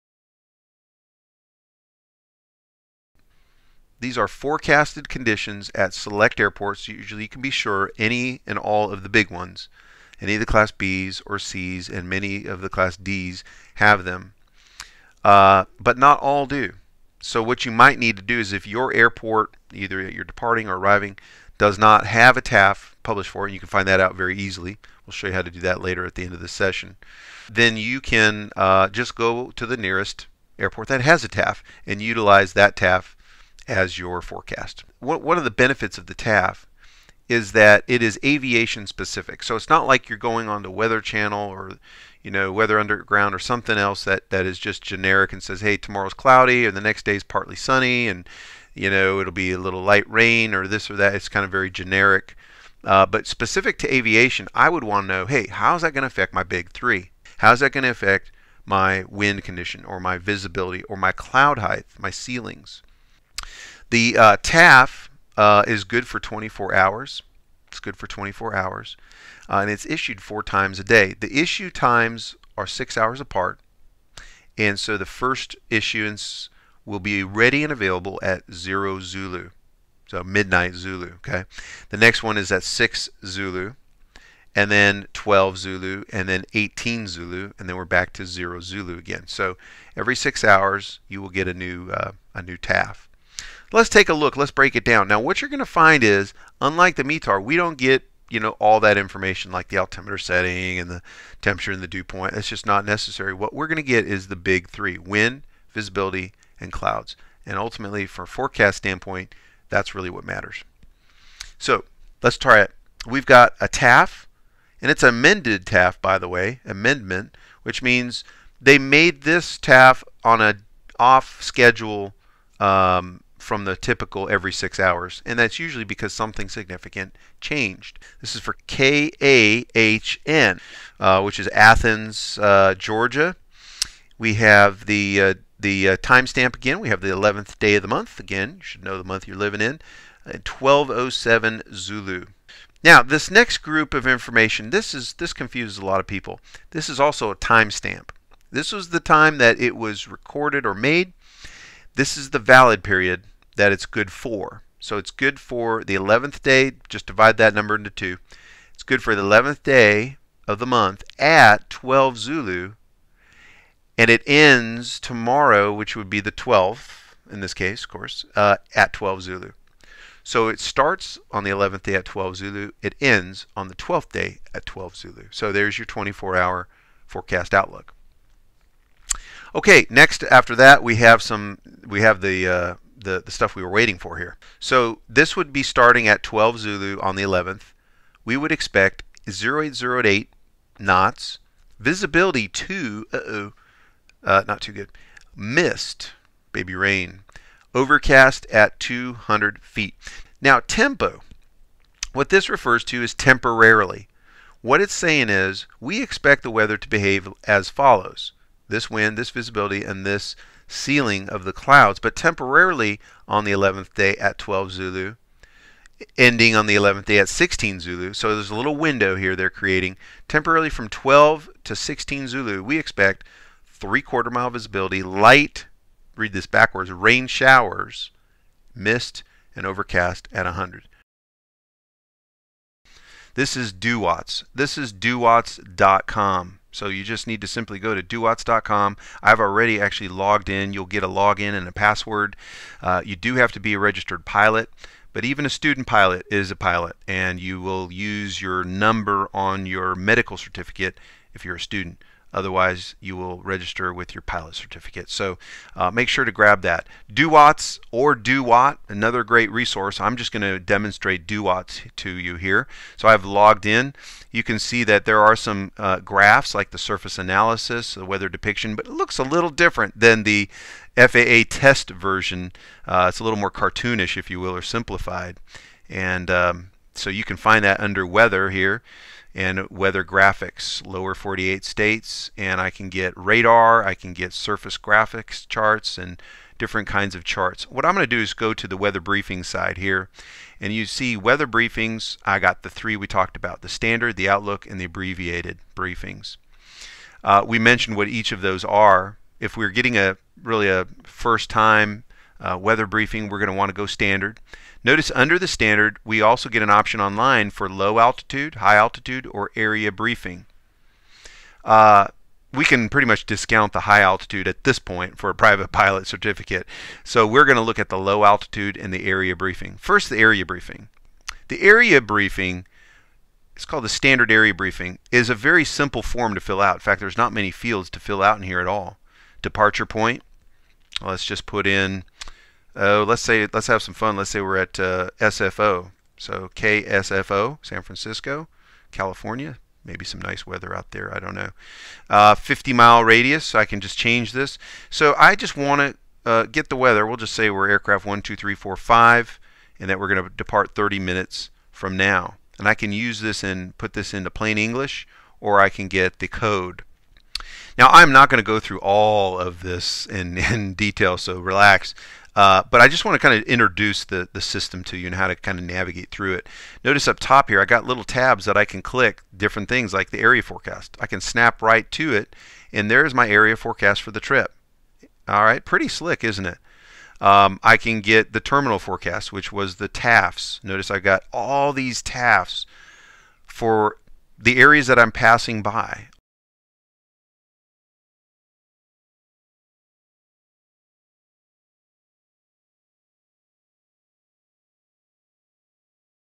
Session 8 - Weather Services
In this sample clip of Ground School USA's Private Pilot Ground School class, you will get an idea of the aviation weather services available to pilots.  Watch excerpts of how to read an aviation forecast called a "TAF" and also see how to access this information online.